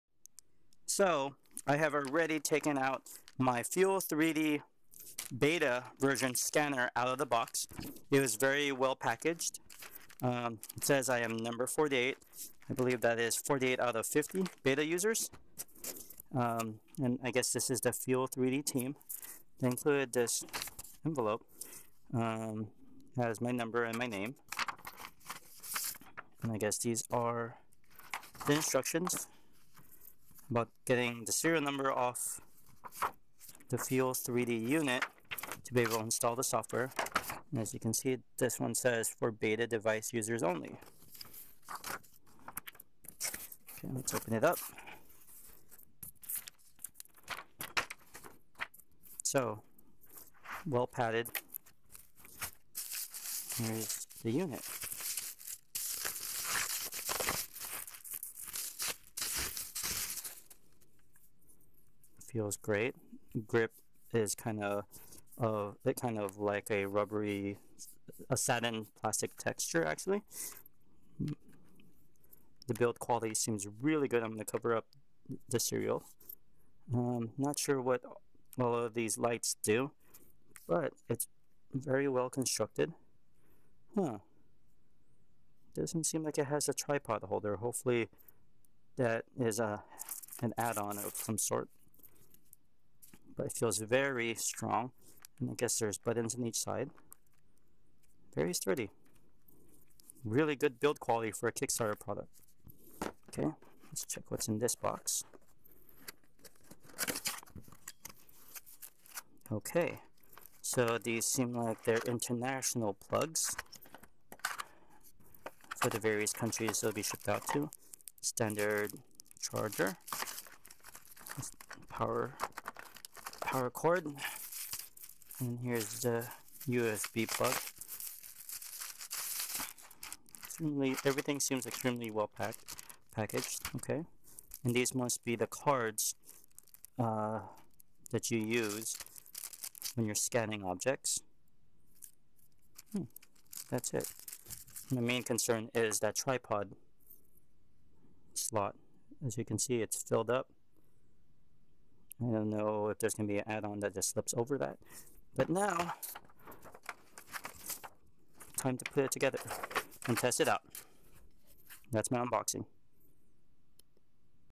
FUEL3D HANDHELD SCANNER BETA EDITION UNBOXING
This is a quick video via Google Glass of my unboxing of my Beta Edition Fuel3D Handheld Scanner